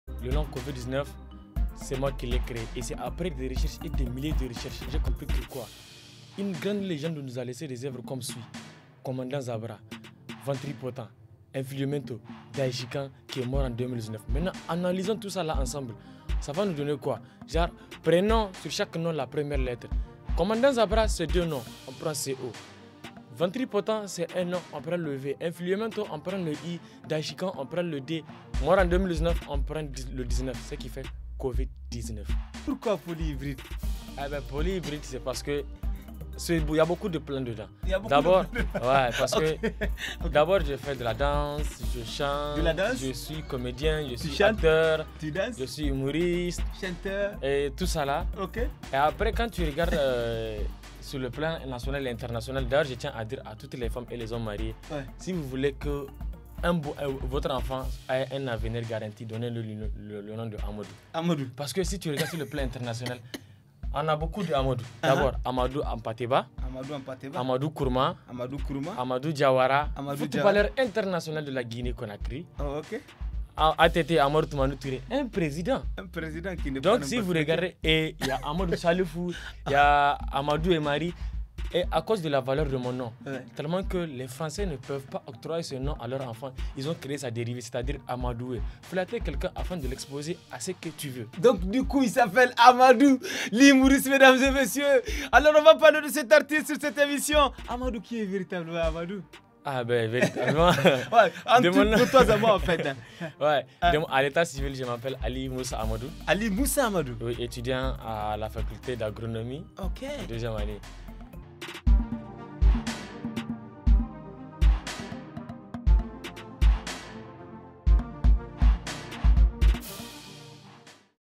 L'artiste s'exprime reçoit Amadou L'homme Poly Hybride
La rubrique "L'artiste s'exprime" reçoit Amadou L'homme Poly Hybride, il est artiste comédien pour qui, le dérivé de son nom est amadoué. Retrouver l'intégralité sur notre site web via ce lien à 18h20.